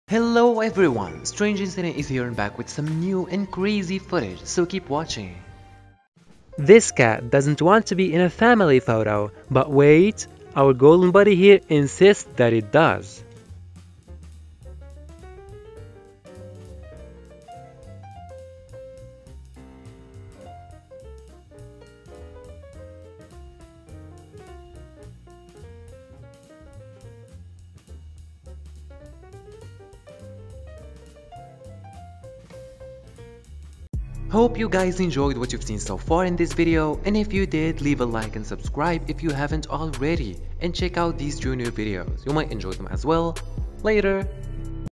Dog Forces Cat To Join Family Photo
This video is a funny video, the main and first clip ⁮where is a golden retriever dog who insists on a cat that it takes a family photo with them while she was looking away and don't want to, this smart buddy figured it out and thought it would be a cute family photo, he even thought about holding the cat from the right spot and not hurting her, what a wonderful dog.
dogs and cats coexist in a heartwarming alliance. Their relationship transcends the stereotypes of natural adversaries, evolving into a testament of companionship and mutual understanding.

For more of the golden and smart dog, you can check him out on Instagram